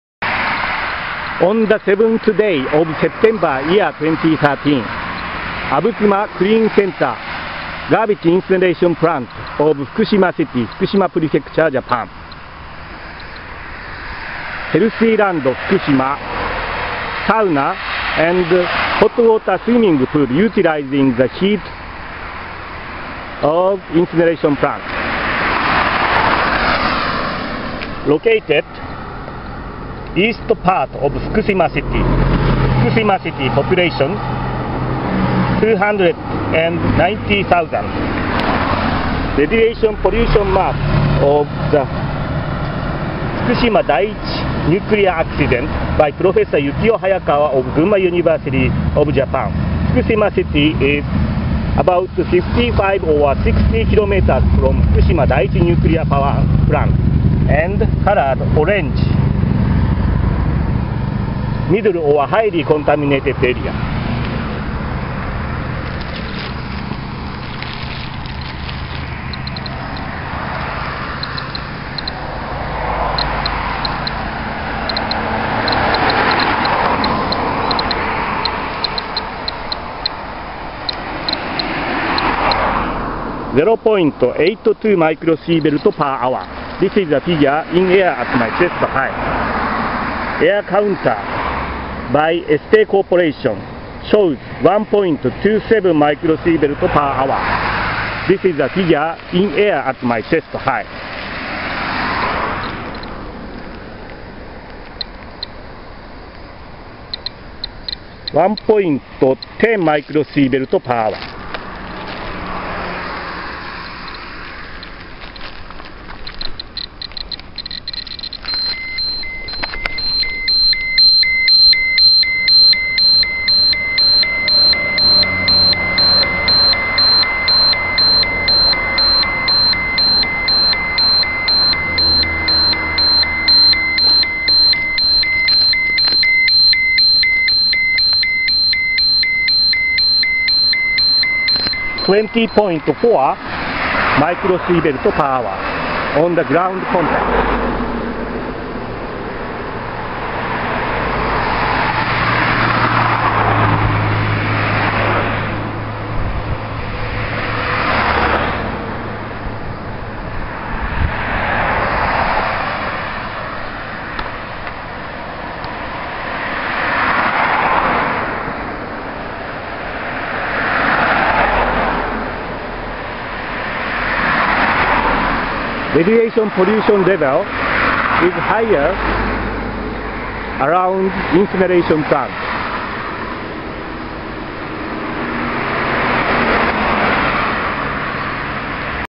Radiation Watch: 20.40 microSv/h, Fukushima city garbage incineration plant, 2013 September
Normal background level is up to 0.2 uSv/hr (microSv/hr)
-rumorecurioso

Cesium boiling point. Aprox.700 degrees Celsius, 1300 degree Fahrenheit.
Garbage incineration easily make Cesium gasification.
Gasification; Cesium cannot be caught by bag filters of the incineration facilities.
Chimney discharges radioactive Cesium.
In wet days, cesium is cooled by rain and fallen immediately beneath.
Serious polutions are brought to the surround of the incineration facilities.
On 7 Sep 2013, I measured radiation around "Abukuma clean center" of Watari area, Fukushima city, Fukushima pref. Japan
I monitored 0.82 to 1.27 micro Sievert per ain air at chest hight, 20.40 on the ground level, on road side mud.
The monitorinig place is 55 km from Fukushima Daiichi Nuclear power plant.
Measuring instrument ECOTEST MKS-05, Ukraine made and Air counter of ST Corp., Japan.
20.40μSv/h 福島市 ごみ焼却場近く歩道の泥 2013.9.7